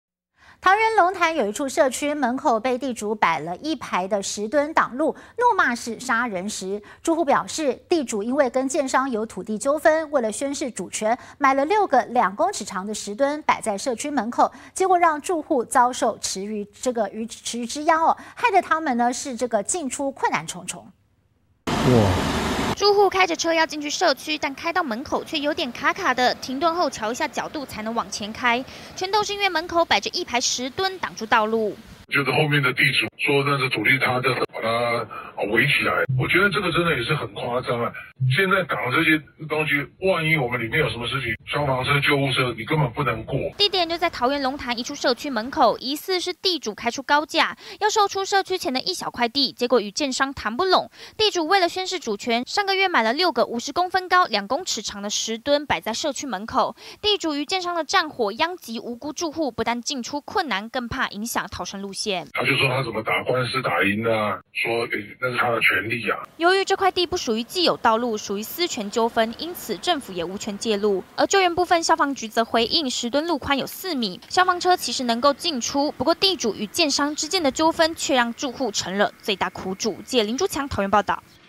地主宣示主權！「石墩陣」擺社區門口 住戶傻眼
桃園龍潭一處社區，門口被地主擺了一排「石墩」擋路，怒罵是殺人石！住戶表示，地主因為跟建商有土地糾紛，為了宣示主權，買了6個、2公尺長的石墩，擺在社區門口，結果讓住戶遭受魚池之殃，害他們進出困難重重。